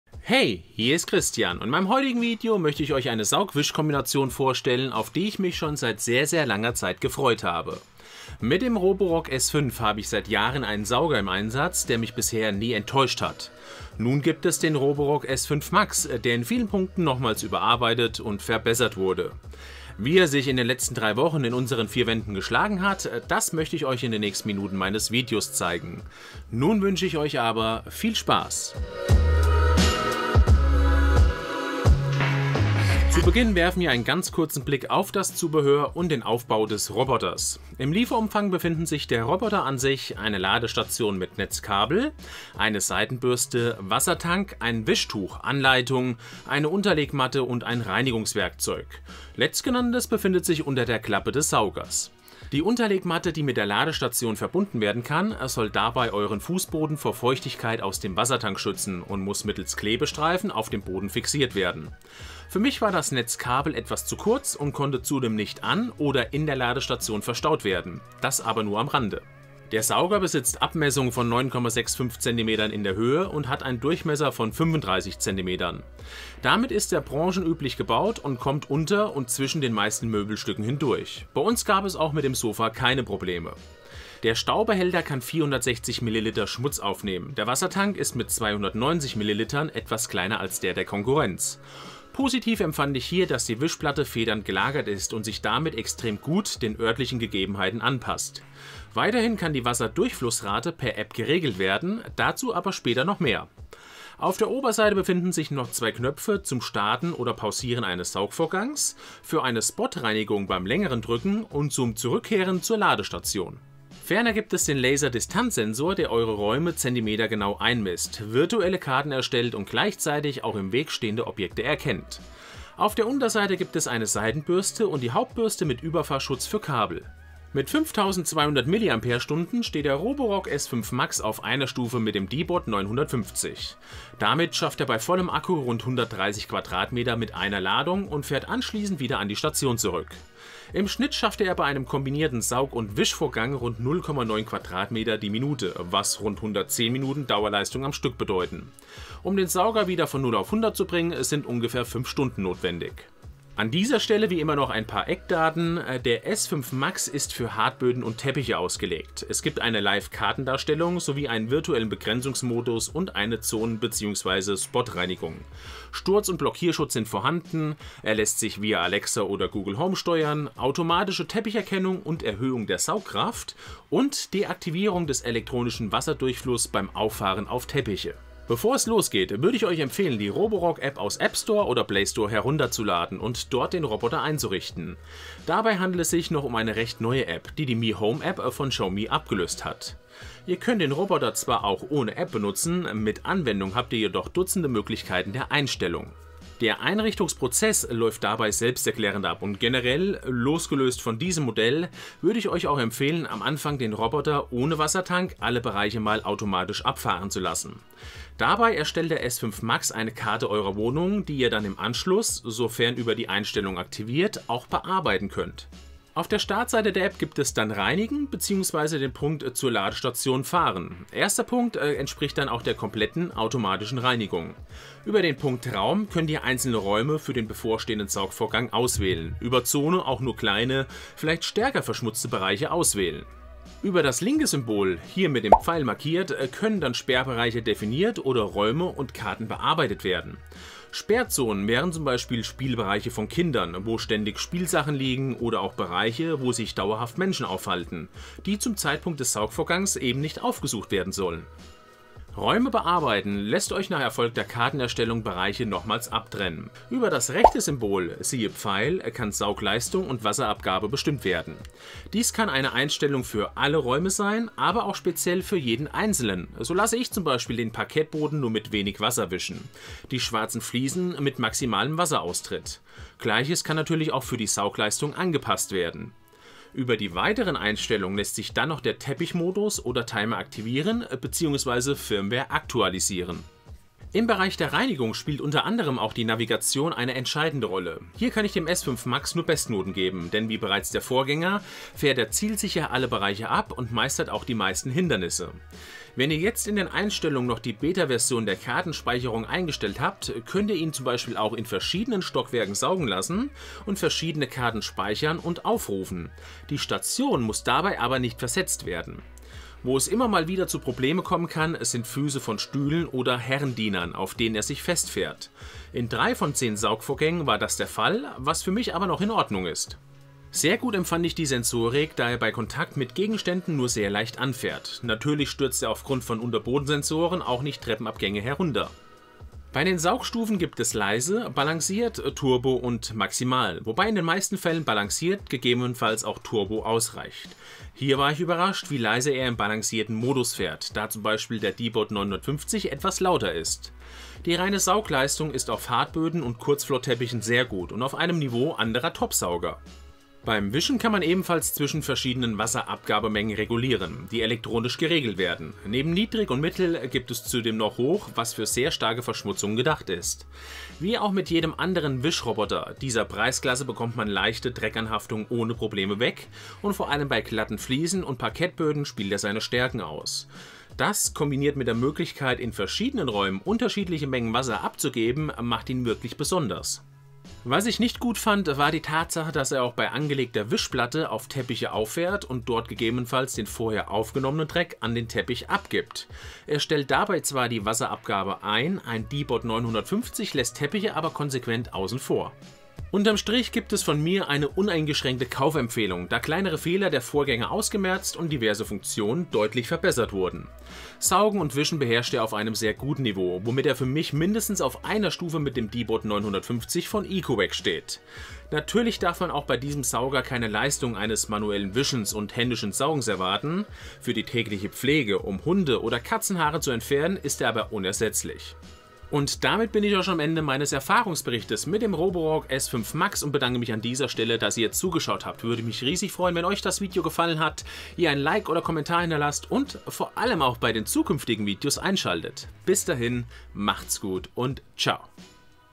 Roborock S5 Max Test | nah an der Perfektion | Saug- und Wischroboter | deutsch
Roborock S5 Max Test - in diesem Video zeige ich euch mein Review zum Saug- und Wischroboter Roborock S5 Max und sage euch wo ich in den letzten drei Wochen positives, vielleicht aber auch negatives gesehen habe.

►Kapitel
00:00 Intro
00:32 Lieferumfang & technische Daten
02:58 Einrichtung
04:43 Reinigung & Saugleistung
06:33 Fazit

✔ Meine Geräte & Ausstattung*:
► Meine Kamera
► Mein Notebook
► Mein Stativ
► Mein Fluid-Head
► Mein Mikrofon
► Meine Beleuchtung